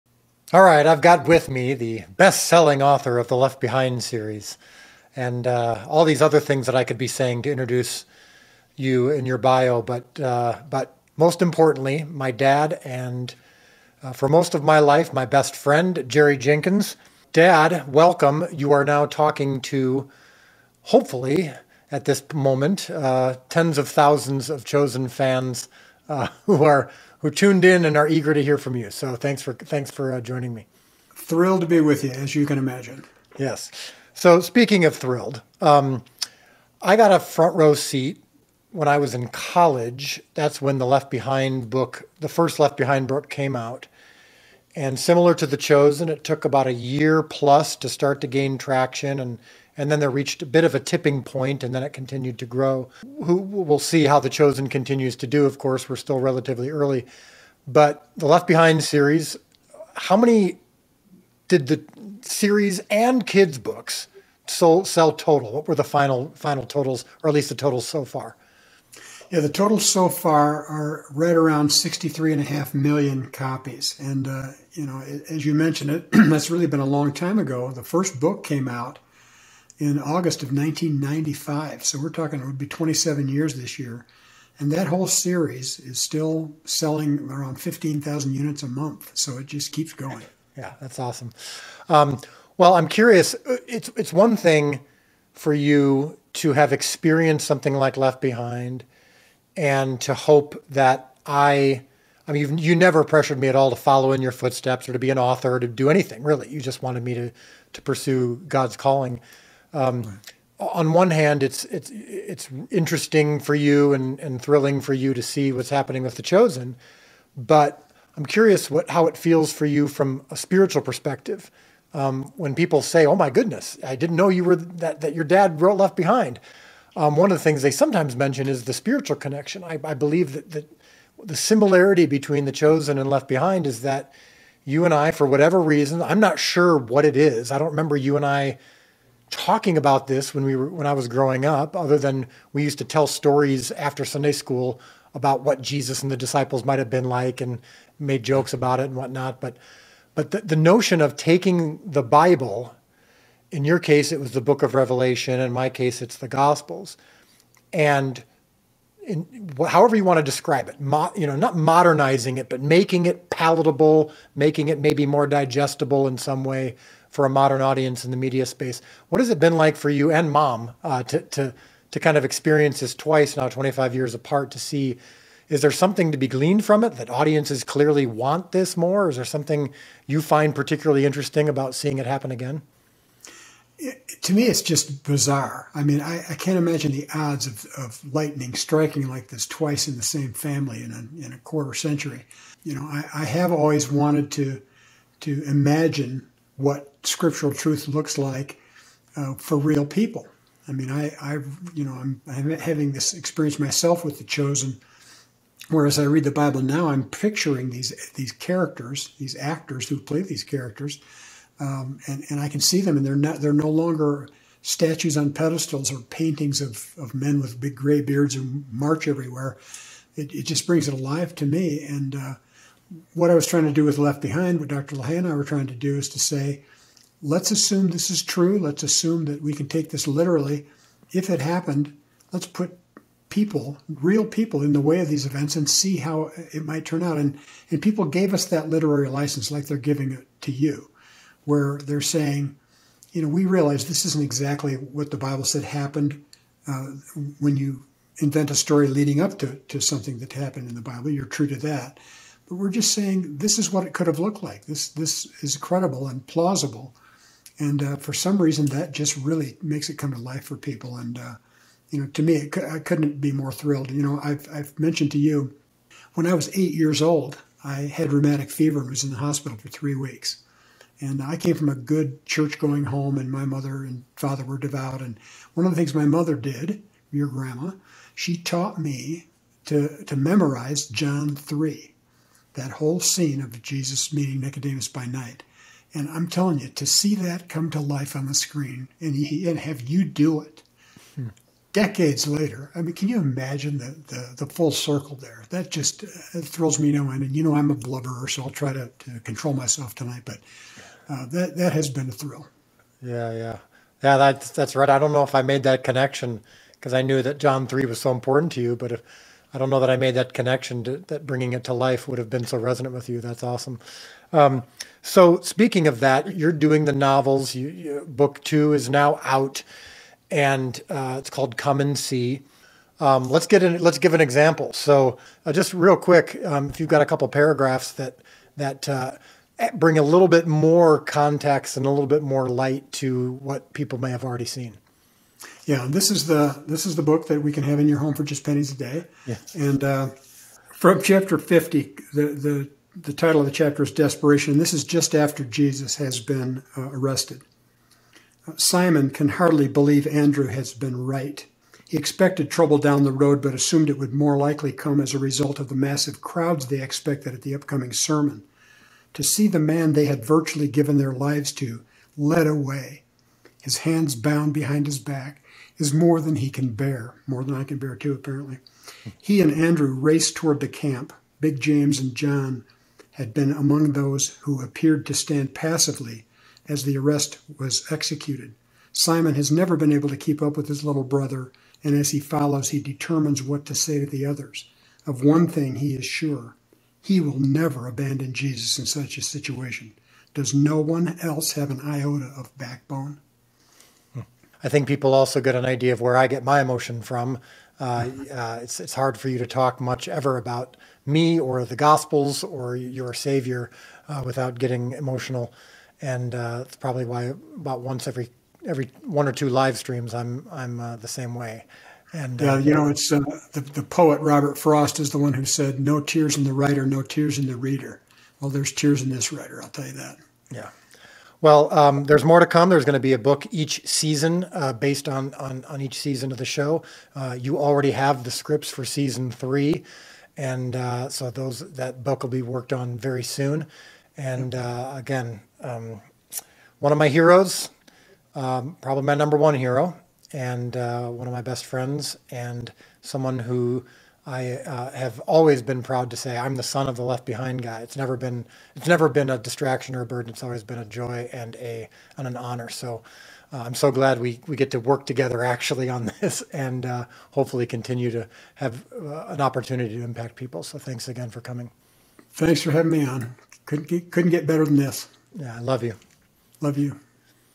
Father & son legacy of The Chosen
You’ve got to check out this fascinating – and frankly, moving – conversation between Dallas and his dad, Jerry. You know Dallas has given us The Chosen. But did you know his dad – Jerry Jenkins – co-authored the Left Behind series, which has sold 63.5 million copies to date? This is eight minutes of two great creators talking shop. And when it’s all said and done, you’ll see that maybe, just maybe, Dallas has finally won the approval of his father.